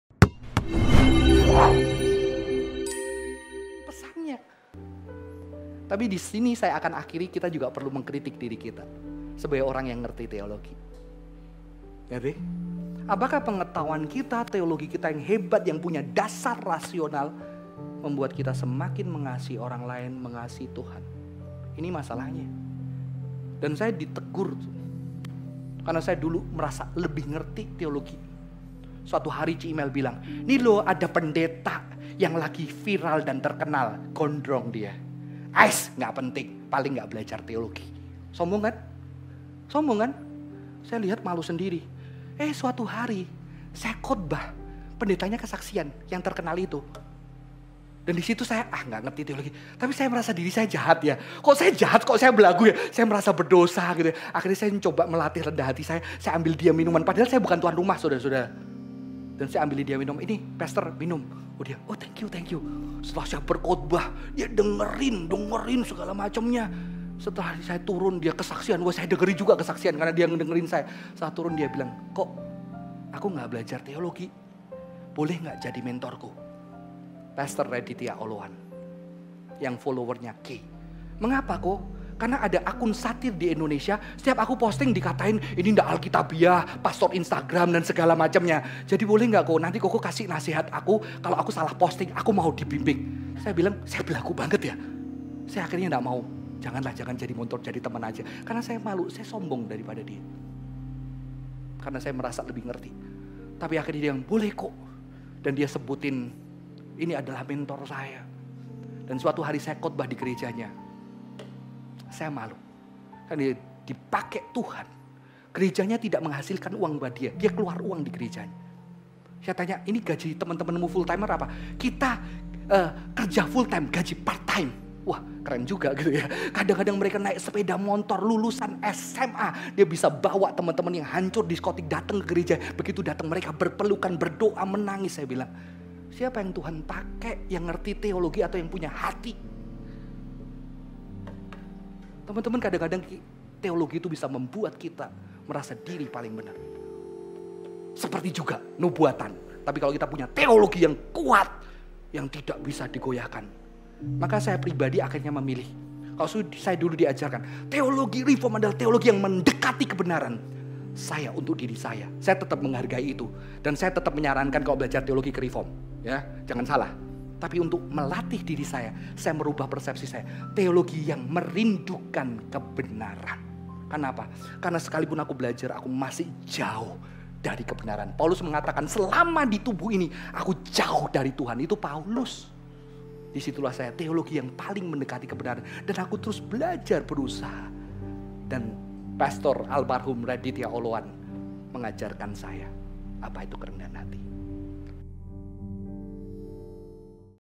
Reformed atau Karismatik yang Membawa Kasih? | Renungan Singkat Kristen (Sermon Snippets)
Gereja reformed atau gereja karismatik, yang terpenting adalah seberapa jauh membawa jemaat kepada Kasih, yaitu Tuhan sendiri. Gereja reformed dengan doktrinnya dan gereja karismatik dengan nubuatnya, keduanya sama-sama bisa menghasilkan jemaat yang sombong dan merasa paling benar. Aliran reformed dikatakan memiliki teologi yang mendekati kebenaran, tetapi biarlah kita boleh memiliki teologi yang merindukan kebenaran. Ini adalah contoh kisah nyata bagaimana pertemuan pastor beraliran reformed dan pastor beraliran kontemporer yang saling belajar rendah hati.

Buat kamu yang penasaran, yuk cek video selengkapnya di playlist Sermon of the Week berjudul Gereja Karismatik dan Nubuatan